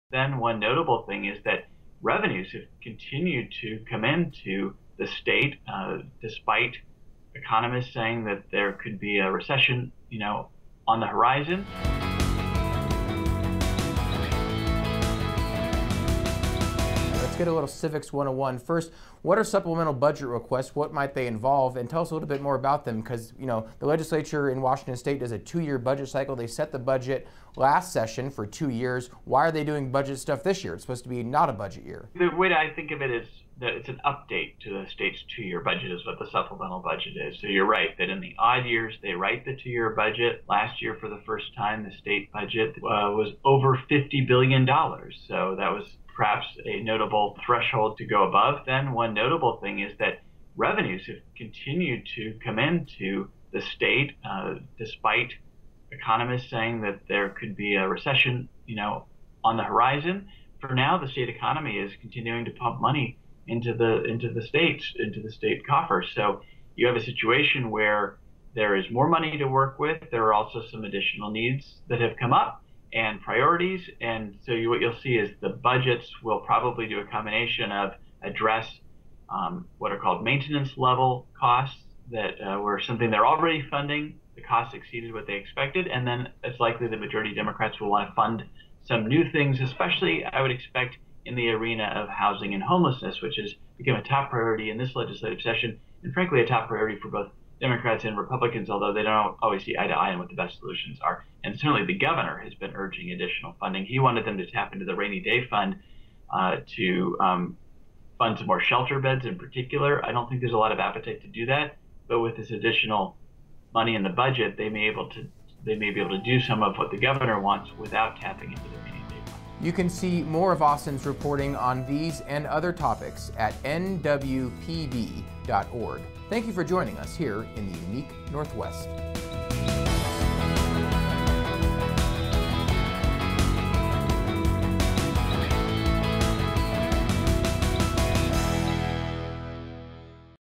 Why Is The Washington State Budget Being Brought Up Again This Year?
Let's get a little civics 101. First, what are supplemental budget requests? What might they involve? And tell us a little bit more about them because, you know, the legislature in Washington is a two year budget cycle. They set the budget last session for two years. Why are they doing budget stuff this year? It's supposed to be not a budget year. Olympia Correspondent Austin Jenkins gives us the answers.

Full Transcript:

The legislative sausage being made or left on the floor as it were. So speaking of legislative sausage though there's still some yet to be made in this session and that also includes what are called supplemental budget requests. Let's get a little civics 101. First, what are supplemental budget requests? What might they involve? And tell us a little bit more about them because, you know, the legislature in Washington is a two year budget cycle. They set the budget last session for two years. Why are they doing budget stuff this year? It's supposed to be not a budget year.

- Yeah, see, the way that I think of it is that it's an update to the state's two year budget is what the supplemental budget is. So you're right that in the odd years they write the two year budget. Last year for the first time the state budget was over 50 billion dollars. So that was perhaps a notable threshold to go above. Now, what the lawmakers do is they come back and some things have changed since then. One notable thing is that revenues have continued to commend to the state despite economists saying that there could be a recession, you know, on the horizon. For now, the state economy is continuing to pump money into the states, in the state coffers. So you have a situation where there is more money to work with there are also some additional needs that have come up and priorities and so what you'll see is the budgets will probably do a combination of address what are called maintenance level costs that were something they're already funding, the costs exceeded what they expected and then it's likely the majority democrats will want to fund some new things, especially I would expect in the arena of housing and homelessness which is become a top priority in this legislative session and frankly a top priority for both democrats and republicans, although they don't always see eye to eye on what the best solutions are. And certainly the governor has been urging additional funding. He wanted them to tap into the rainy day fund to fund some more shelter beds in particular. I don't think there's a lot of appetite to do that but with this additional money in the budget they may be able to do some of what the governor wants without tapping into the rainy day fund.

- You can see more of Austin's reporting on these and other topics, at NWPB.org. Thank you for joining us here in the Unique Northwest.